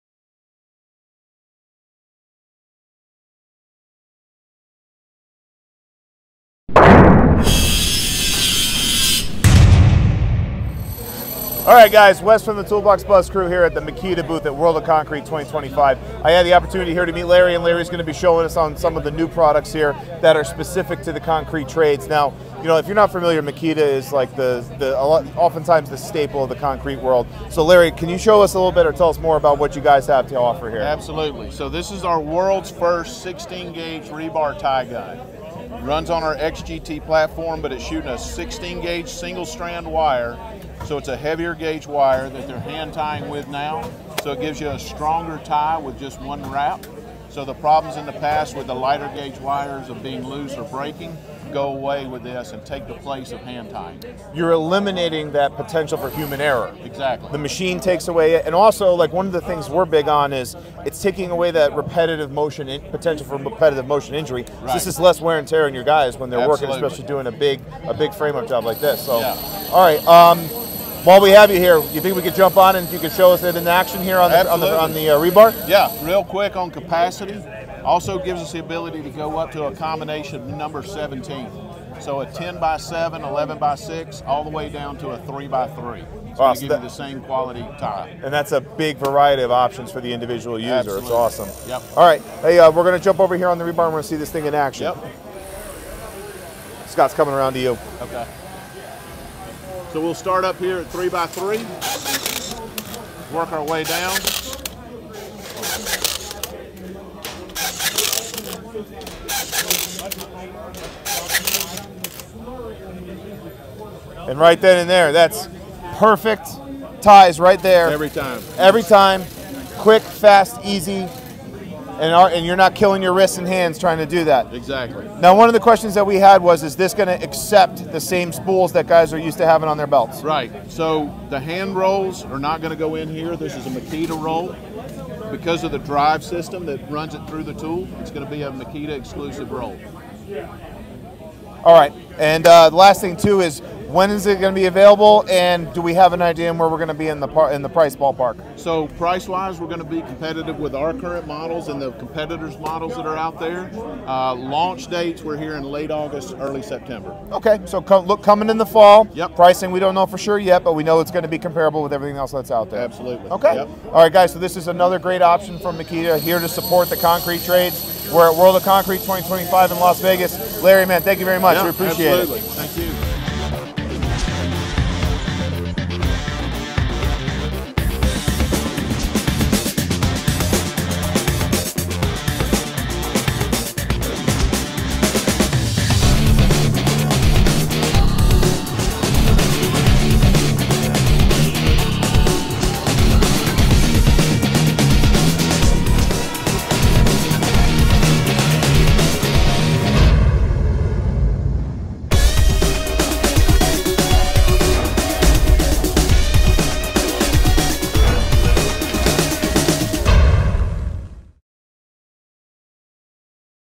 Makita 40V XGT 16 GA. Rebar Tying Tool | NEW 2025
NEW 40V max XGT® 16 Ga. Rebar Tying Tool - The expanding 40V | 80V XGT® System offers more tools and equipment for concrete applications, and a key addition is the new 40V max XGT® Brushless 16 Ga. Rebar Tying Tool. It is the world’s first 40V max 16 gauge wire rebar tying tool. The GRT01 delivers up to 65% stronger ties, up to 3,600 ties per charge, and deep tying capacity (#9 x #8 rebar).